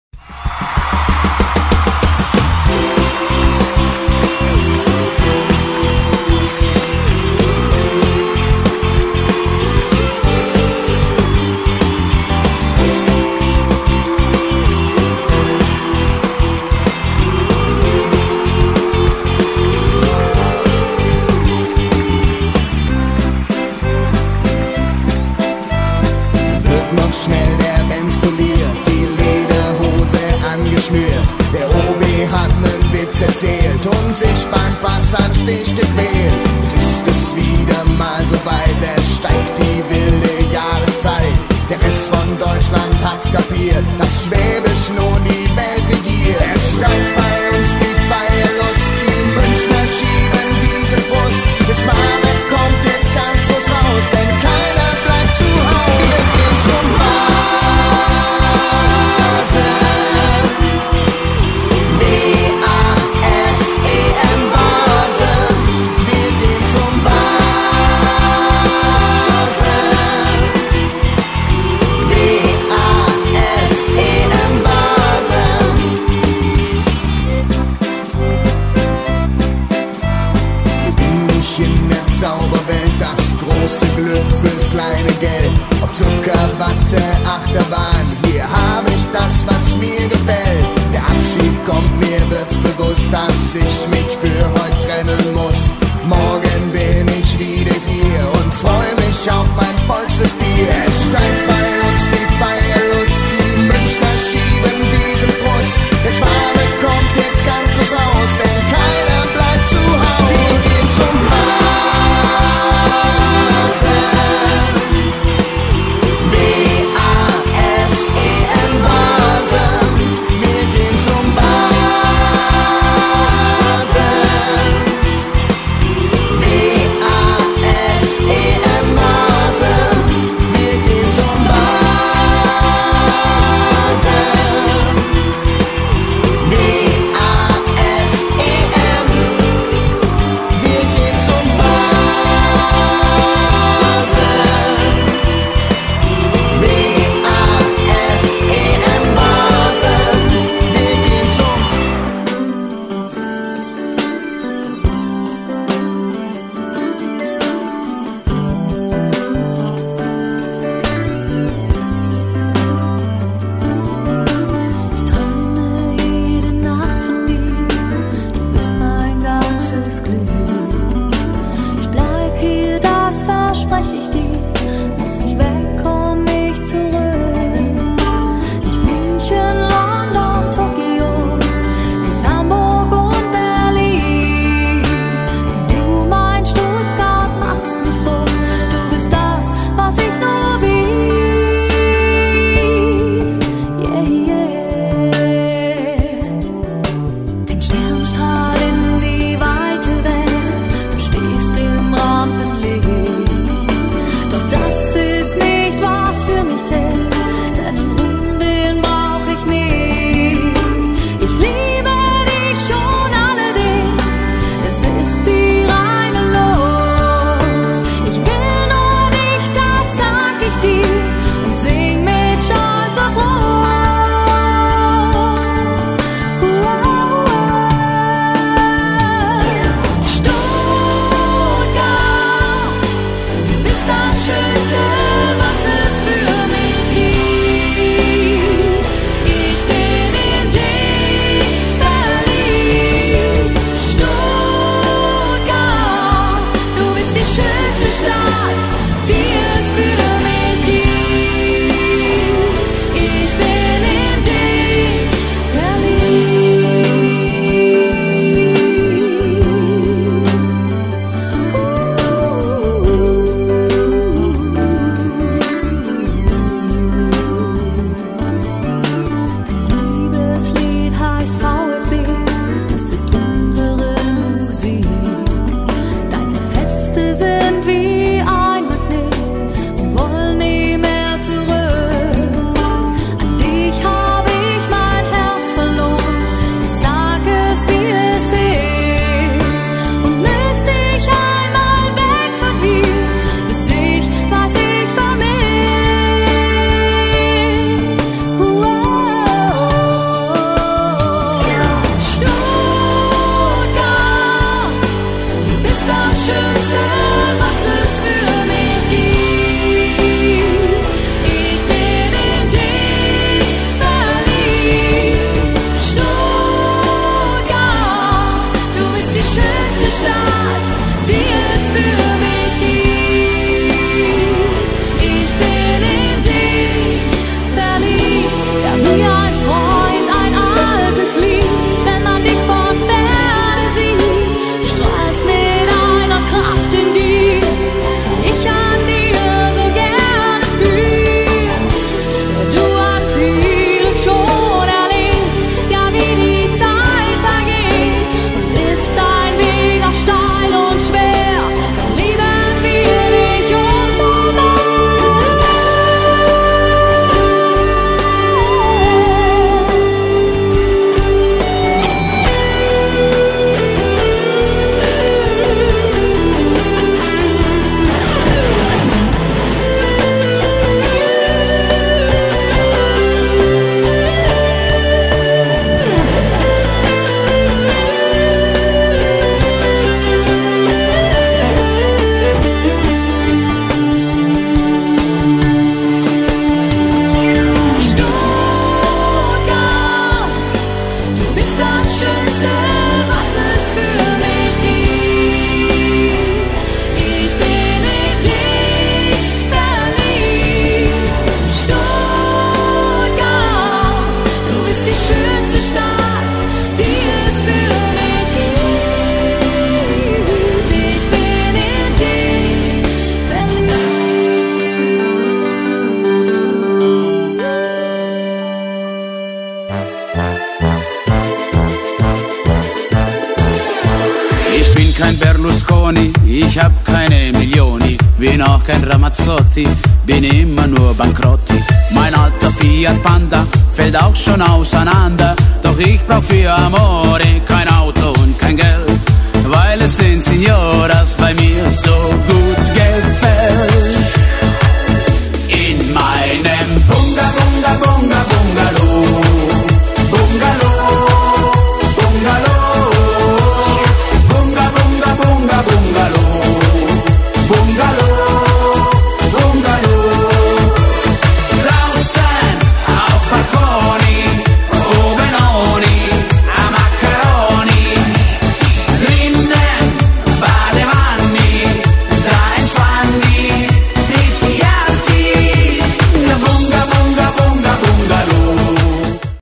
Di 21.04.2015
Stimmung im Grandls Hofbräuzelt am Freitag den 21. April 2014 mit den den WIPS und dem Hofnarr Luigi.